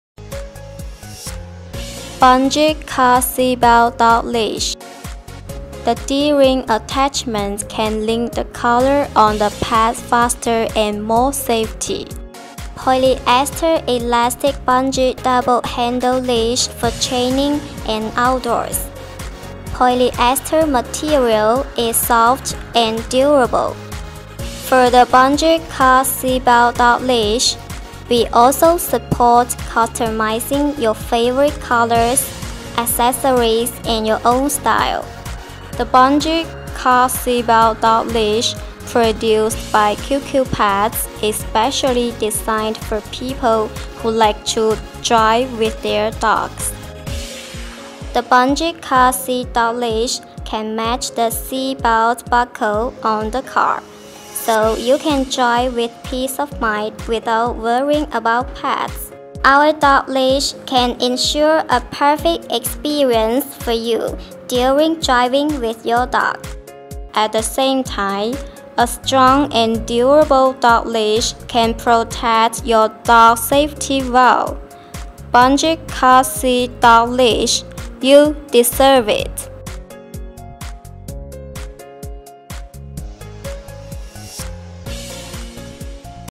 Bungee Car Seat Belt Dog Leash
The D-ring attachment can link the collar on the pet faster and more safety.Polyester elastic bungee double handle leash for training and outdoors.Polyester material is soft and durable.For the bungee car seat belt dog leash, we also support customizing your favorite colors ,accessories and your own style.The bungee car seat belt dog leash produced by QQPETS is specially designed for people who like to drive with their dog.The bungee car seat dog leash can match the seat belt buckle on the car, so you can drive with peace of mind without worrying about pets.Our dog leash can ensure a perfect experience for you during driving with your dog.At the same time,a strong and durable dog leash can protect your dog's safety well .Bungee car seat dog leash, you deserve it.